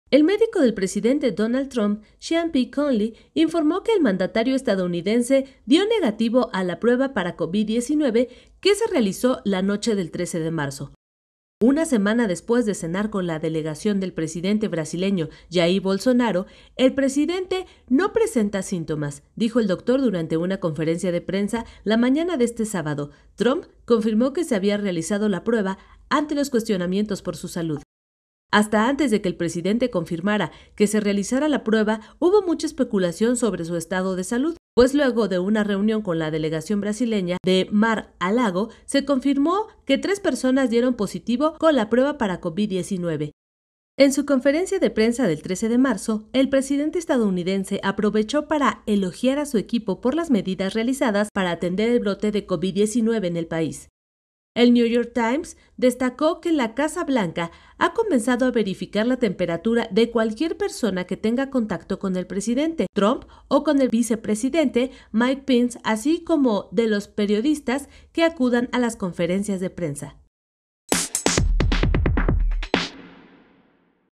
Trump da negativo en prueba por Covid 19
El médico del presidente Donald Trump, Sean P. Conley, informó que el mandatario estadounidense dio negativo en la prueba para Covid-19 que se realizó la noche del 13 de marzo.